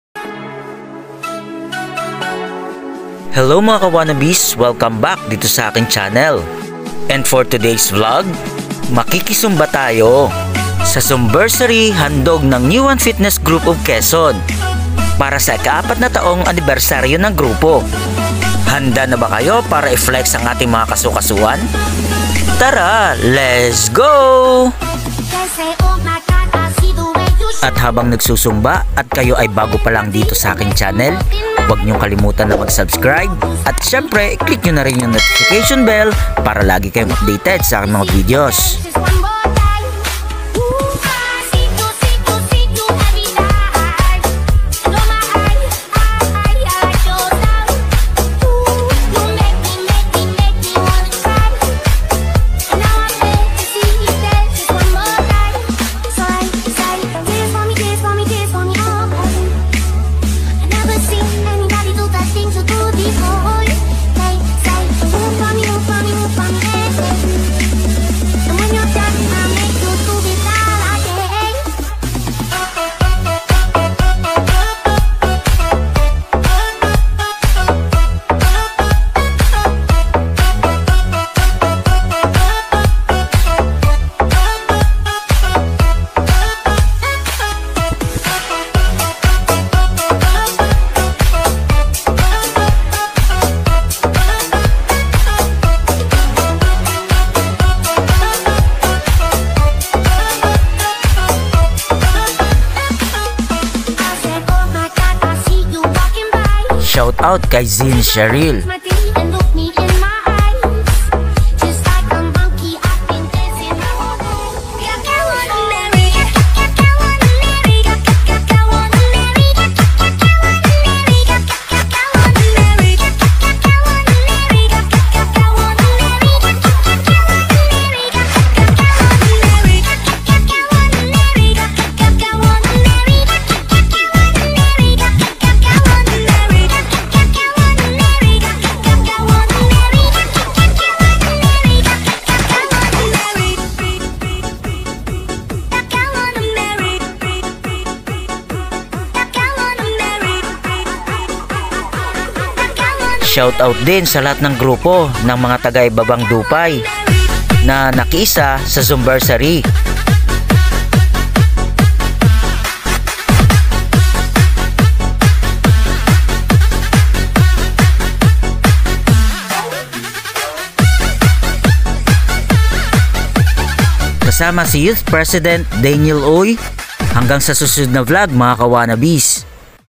ZUMVERSARY BY N1FGQ - One Day Vlog
4th Anniversary of New One Fitness Group of Quezon
Thank you so much Brgy. Ibabang Dupay
Zumversary